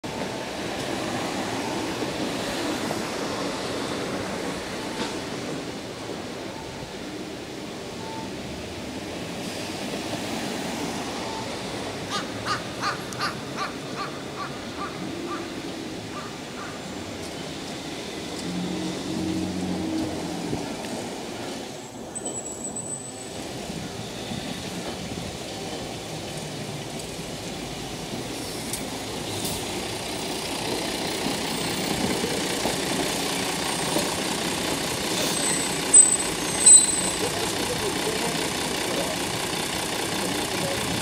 札幌市電8510形 すすきの仮停留場発車 Sapporo Streetcar Type 8510
札幌市電のループ化工事のため月寒通の約100メートル西側に移設されたすすきの停留場から西4丁目行き電車が出発する。
Sapporo City Transportation Bureau Yamahana Line tramcar will depart from Susukino tram stop, Hokkaido, Japan.